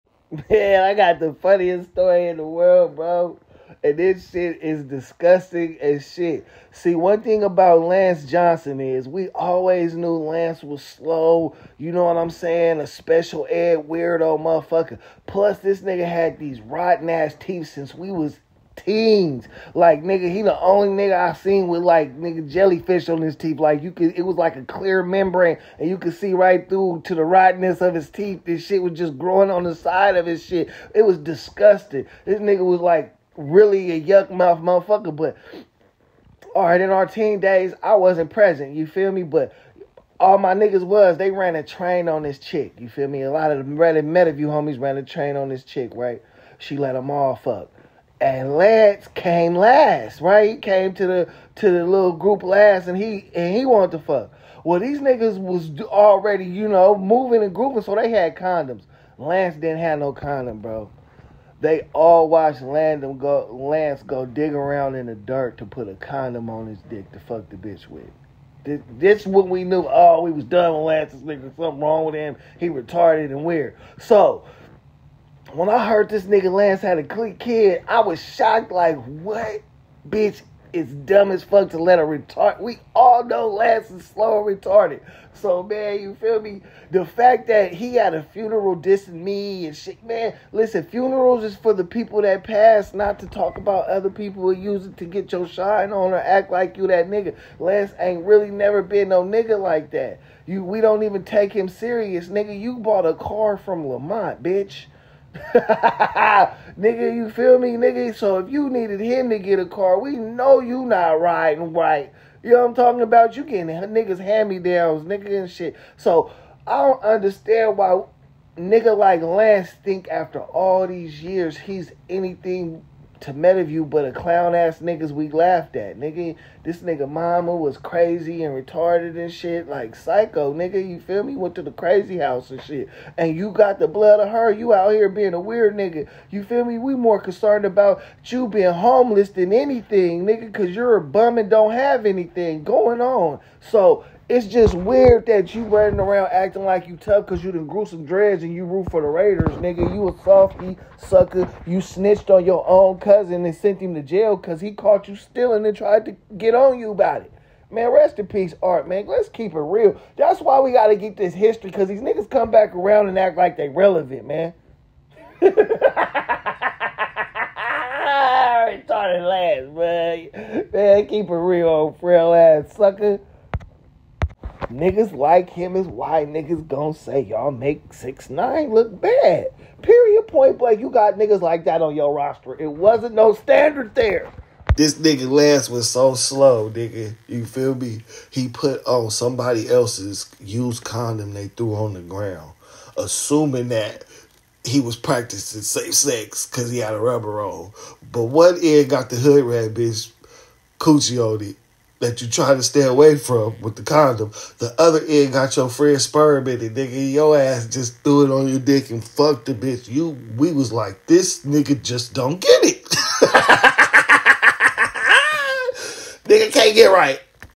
Lying Ass Lanz Make 69 Look Bad Using Other Men Used Condoms
The Only Thing Lanz Johnson Is Known For is Used Condoms